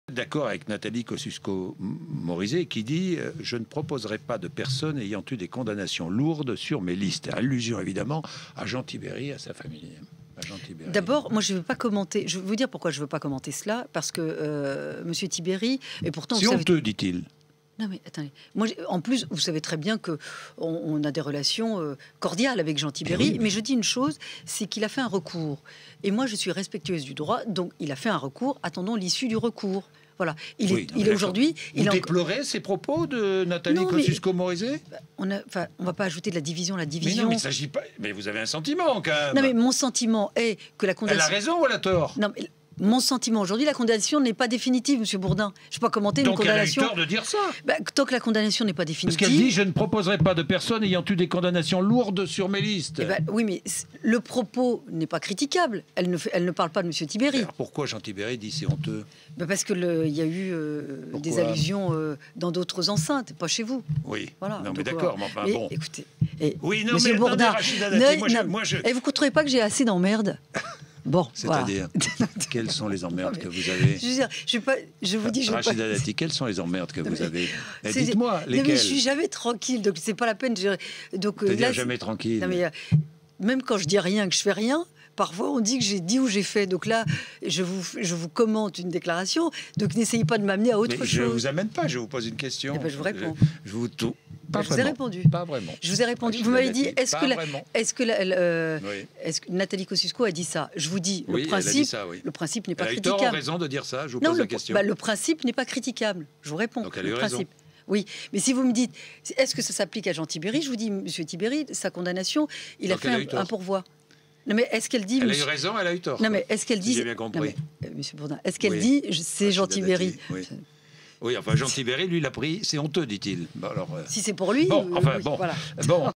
Rachida Dati: "Vous ne trouvez pas que j'ai assez d'emmerdes comme ça" - 05/04
Vous ne trouvez pas que j'ai assez d'emmerdes comme ça", a déclaré Rachida Dati à Jean-Jacques Bourdin qui lui demandait de commenter une déclaration de Nathalie Kosciusko-Morizet, sa rivaleUMP pour les municipales à Paris.
La députée européenne, ancienne garde des Sceaux et candidate UMP à la mairie de Paris, était l'invitée de Jean-Jacques Bourdin ce vendredi.